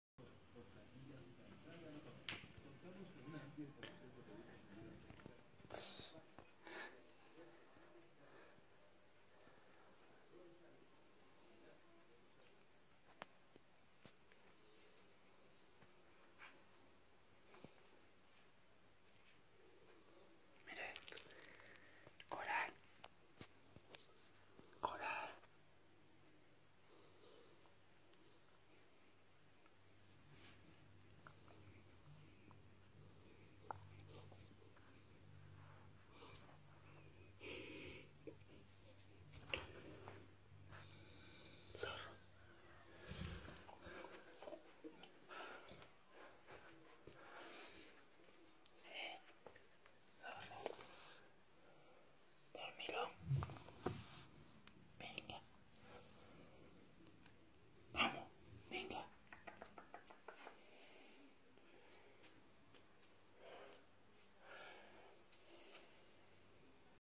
Coral 🐕 y Manu 🐶 duermen tranquilos 💤
Coral 🐕  y Manu 🐶  duermen tranquilos 💤  💤  porque saben que yo los protejo ❤❤ ... y sé 😉  que ELLOS 🐕  🐶  cuidan de mí

🔝 SomosCANARIOS⚪💙 💛 de pura raza